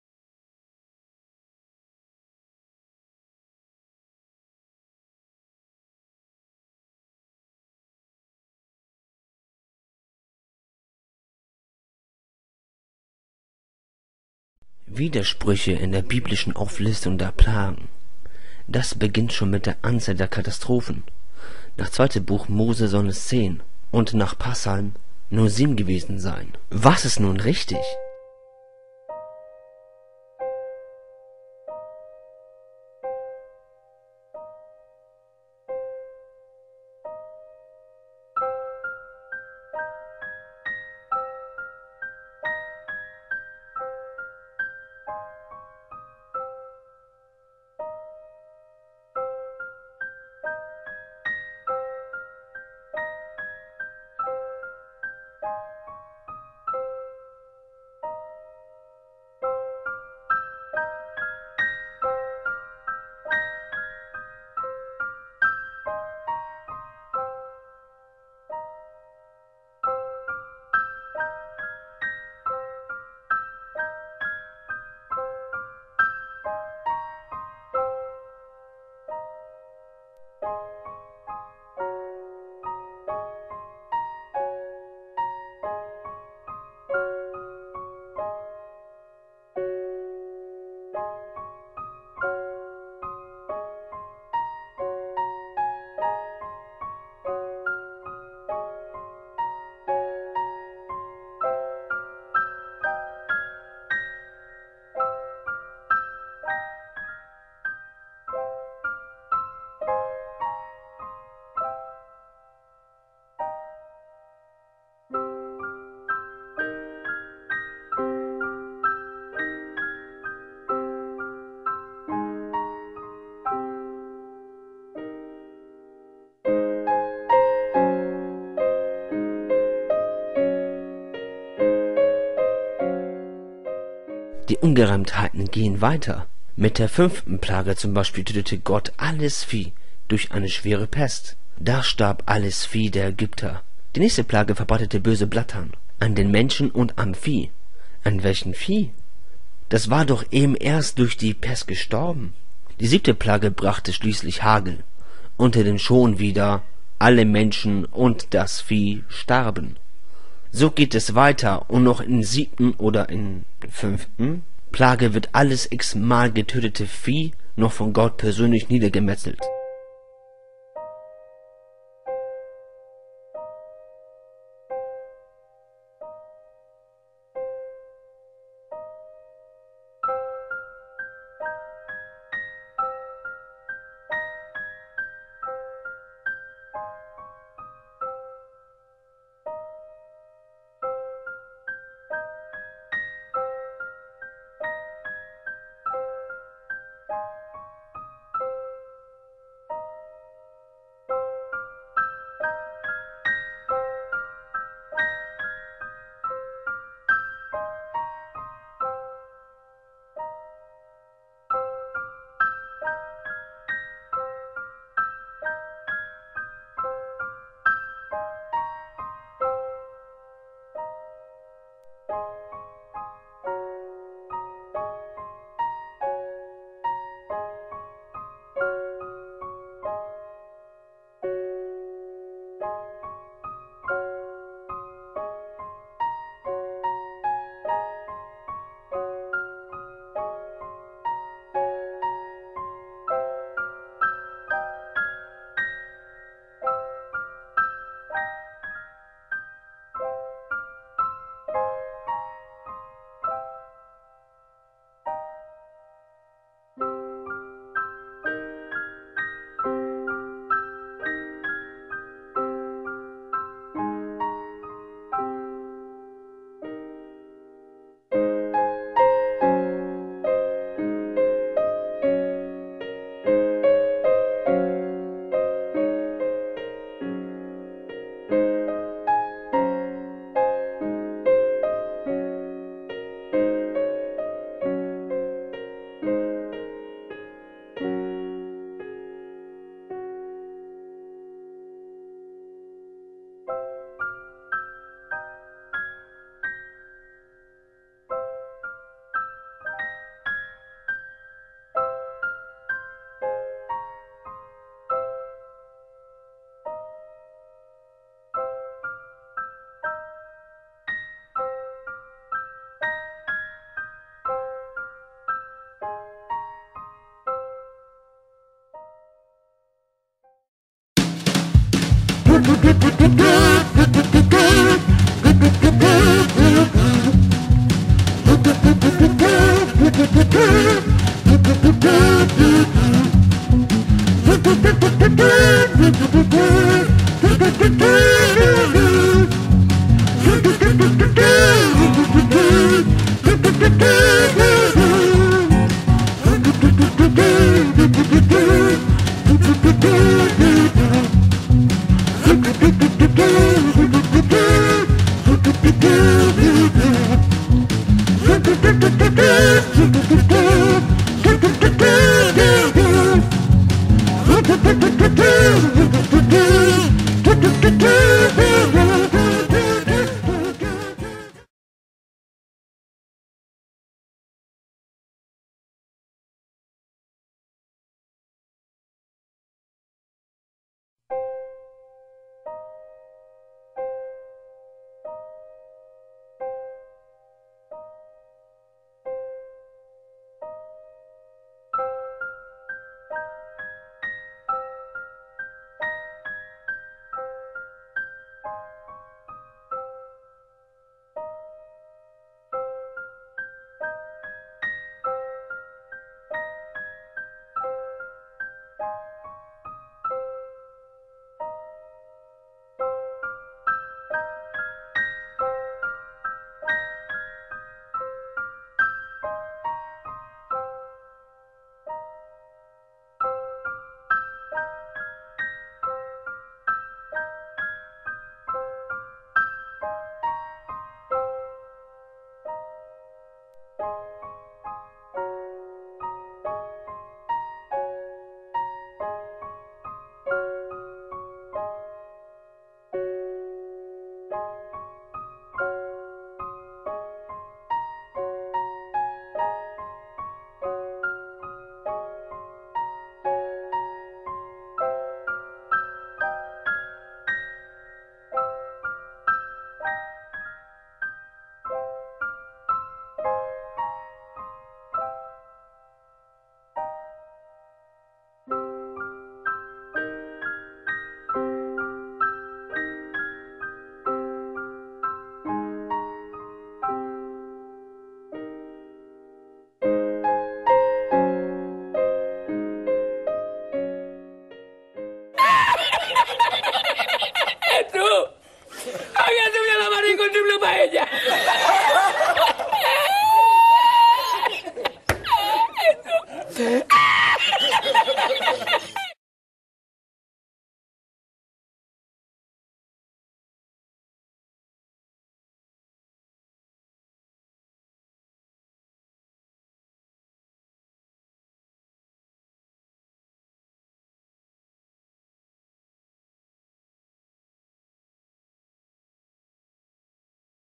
10 oder 7 Plagen in der Bibel? Wie oft starb das Vieh der Ägypter? - Starmoon Islam widerlegt!
Zehn oder sieben Plagen in der Bibel? Wie oft starb das Vieh der Ägypter? - Starmoon Islam widerlegt! (2.Mose 7-12) & (Psalm 105:26-36)

Antwortvideo an Starmoon Islam der dieses "Argument" von atheistischen Webseiten kopiert hat und sich damit blamiert. § 51 UrhG Zitate

RE: Die 10 oder 7 Plagen ? Widerspruch in der Bibel